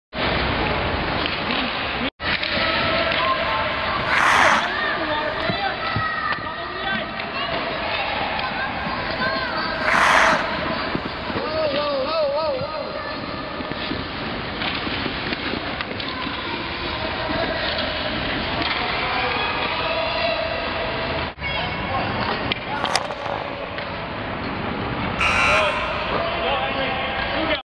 Stanley's First Ice Hockey Mighty Mights
City Ice Mighty Mites! March 2009
Work in progress...first bit...